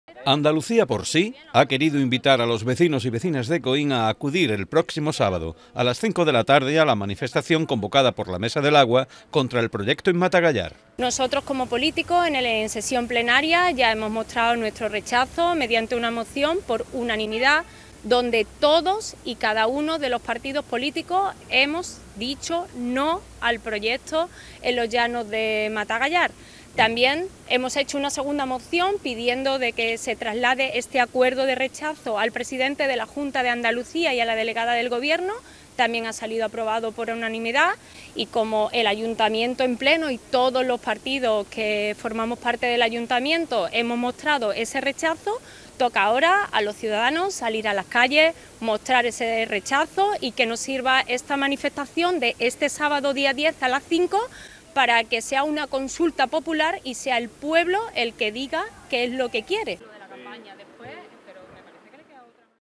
INVITACIÓN ANDALUCÍA POR SÍ MANIFESTACIÓN
Andalucía por sí ha querido invitar a los vecinos y vecinas de Coín a acudir el próximo sábado a las cinco de la tarde a la manifestación convocada por la mesa del agua contra el proyecto en Matagallar.